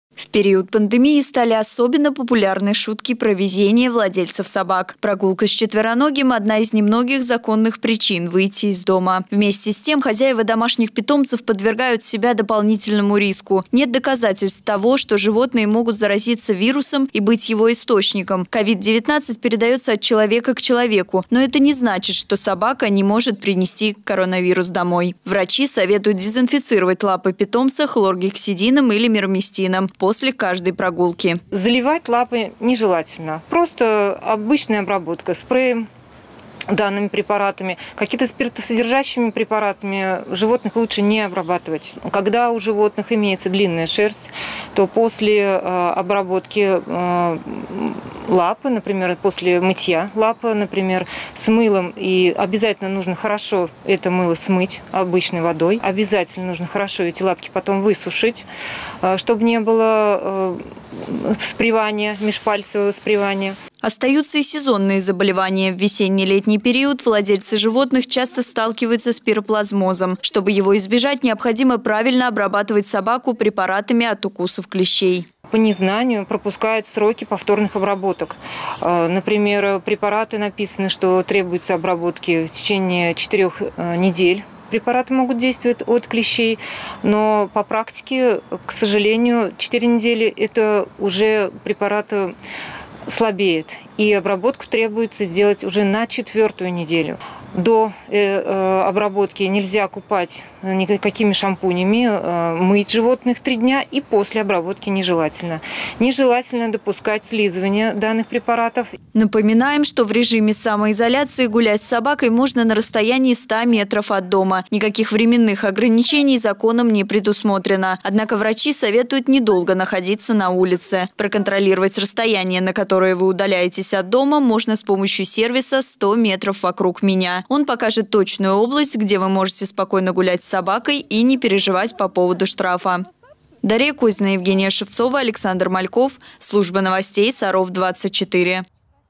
Безопасная прогулка
Во время пандемии желательно сидеть дома. Но во многих семьях живут собаки, которым необходимы ежедневные выгулы. Как снизить риск заражения коронавирусом?